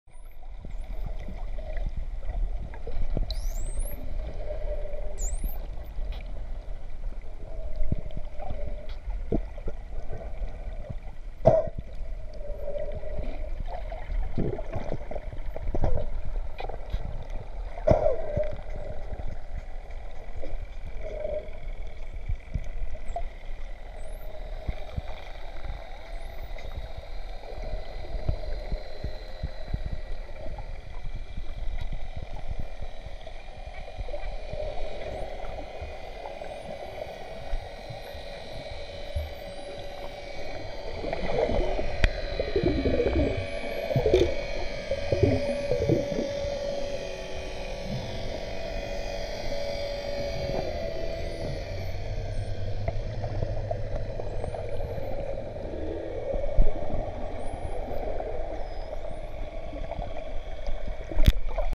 Aperçu d'un moment magique
Croisières à thèmes et nage en conscience avec les dauphins sauvages de Sataya / Egypte